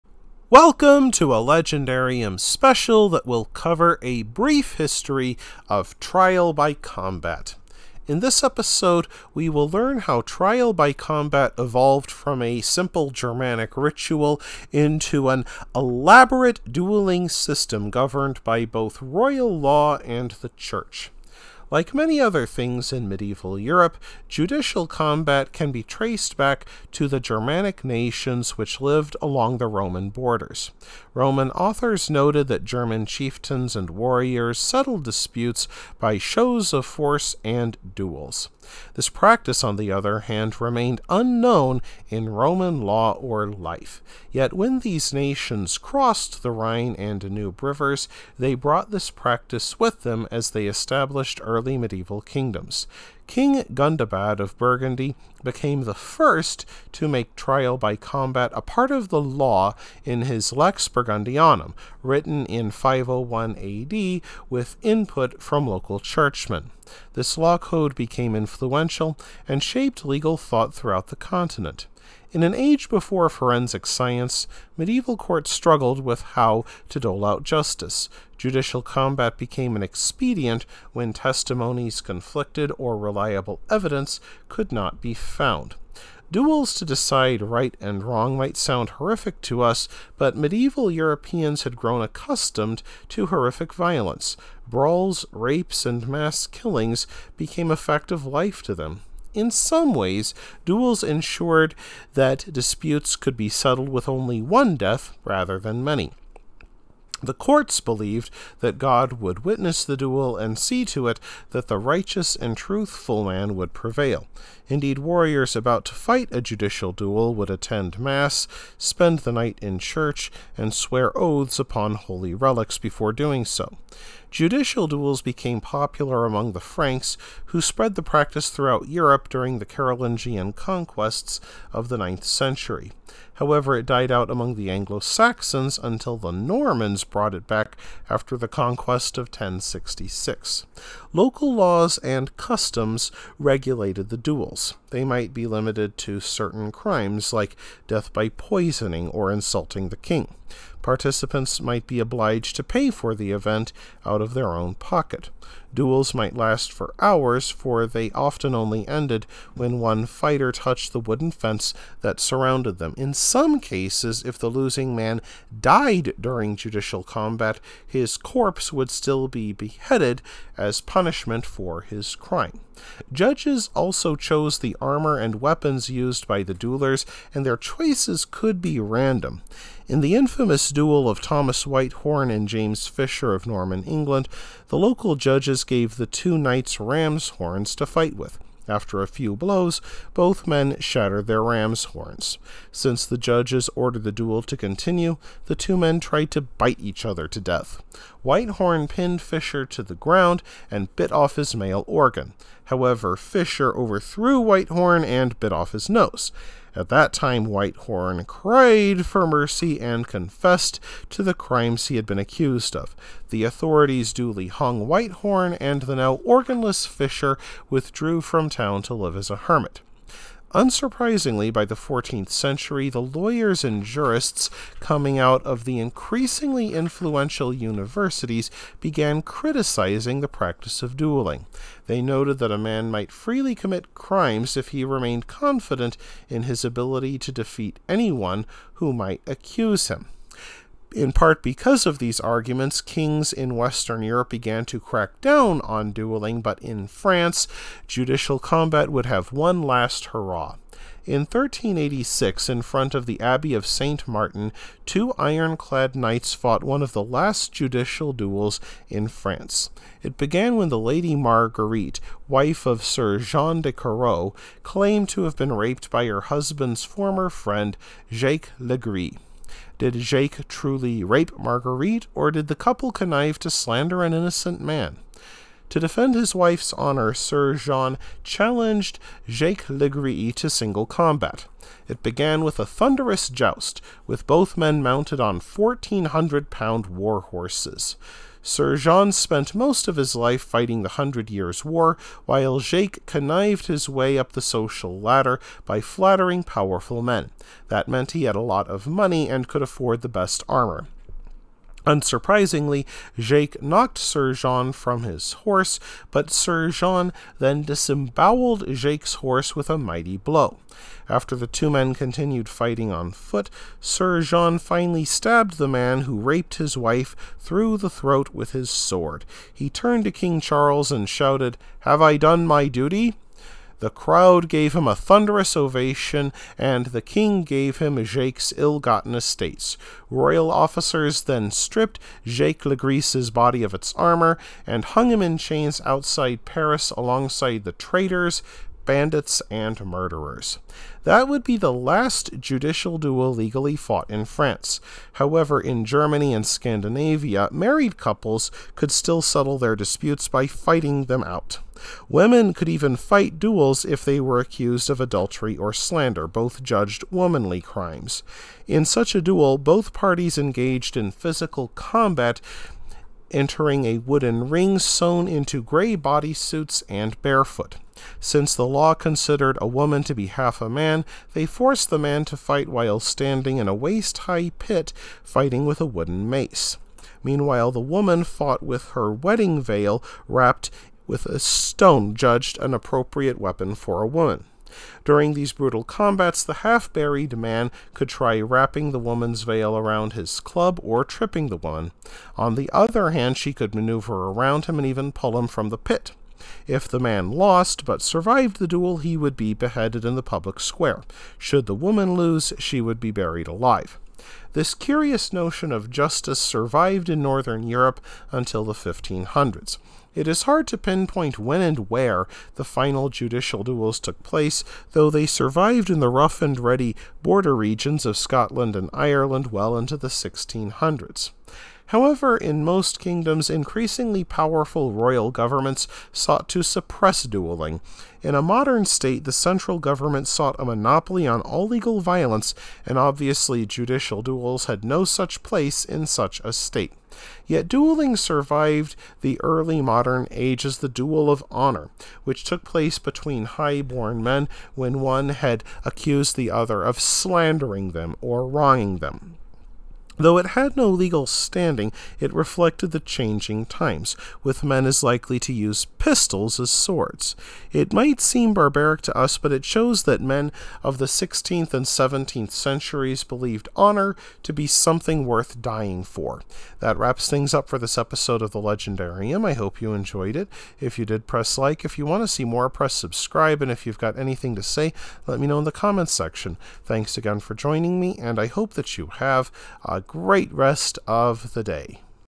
A Brief History of Trial by Combat
One of the most romanticized images of the Middle Ages is trial by combat. Yet, unlike what you see in the movies, knights might be forced to duel with rams' horns, pay for staging the duel, or even fight while half-buried in the ground!